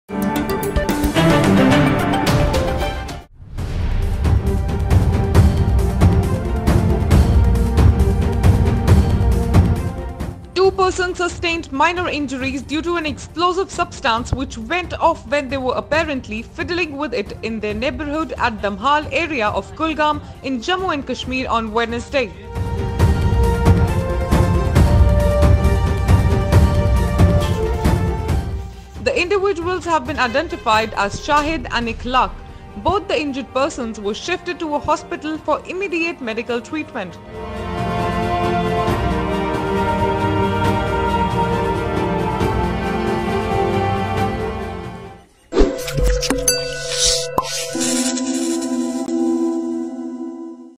2 persons injured after fiddling with explosive substance
JAMMU LINKS NEWS | Two persons sustained minor injuries due to an explosive substance which went off when they were apparently fiddling with it in their neighbourhood at Damhal area of Kulgam in Jammu and Kashmir on Wednesday. The individuals have been identified as Shahid and Ikhlaq. Both the injured persons were shifted to a hospital for immediate medical treatment.